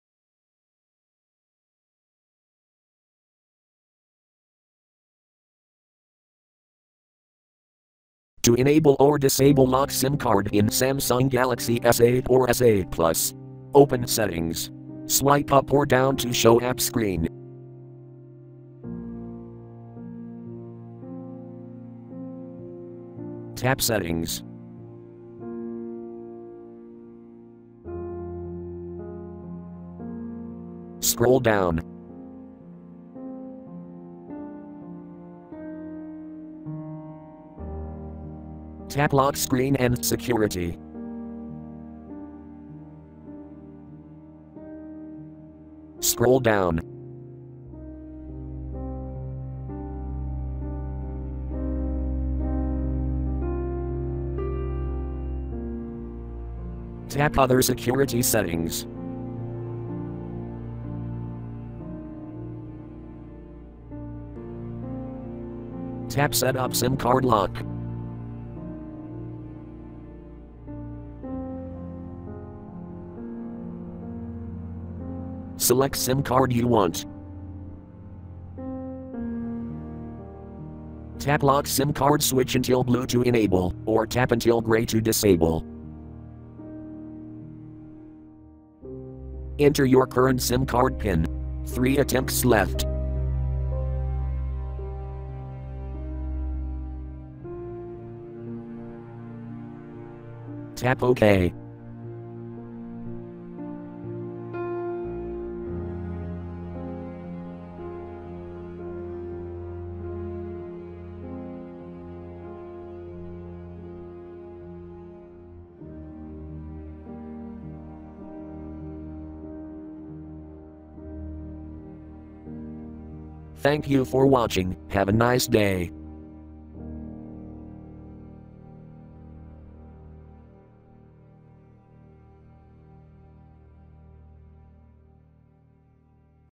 Android Nougat : How to Enable or Disable Lock SIM card in Samsung Galaxy S8 or S8+
This video show How to Enable or Disable Lock SIM card in Samsung Galaxy S8 or S8+.  In this tutorial I use Samsung Galaxy S8 SM-G950FD International version with Android 7.0 (Nougat).